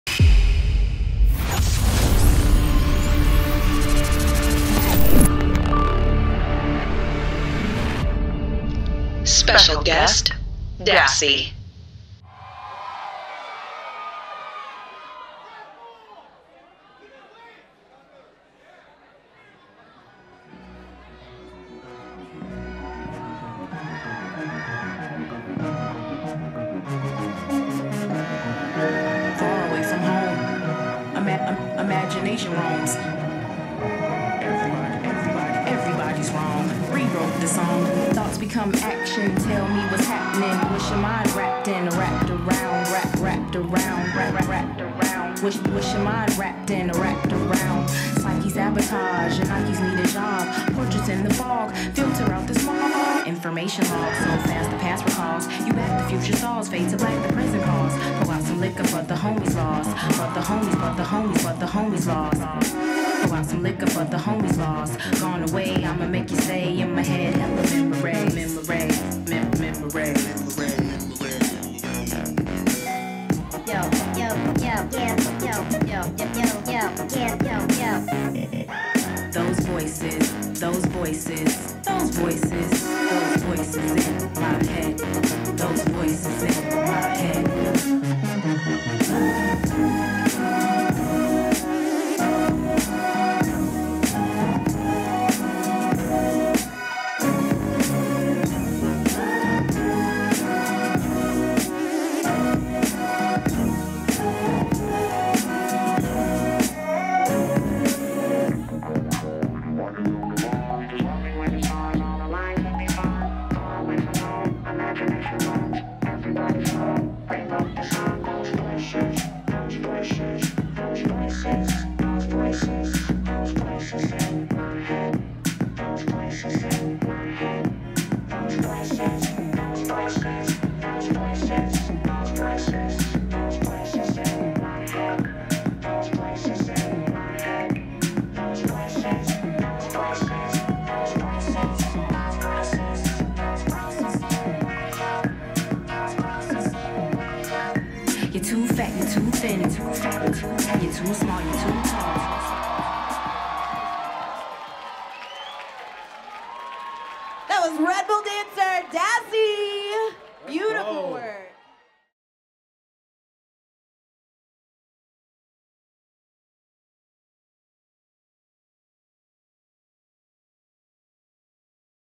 Urbanite XXII: Chicago | Dassy
Dassy @ Urbanite XXII: Chicago, 4.6.19 @ Metro Chicago (uncensored audio)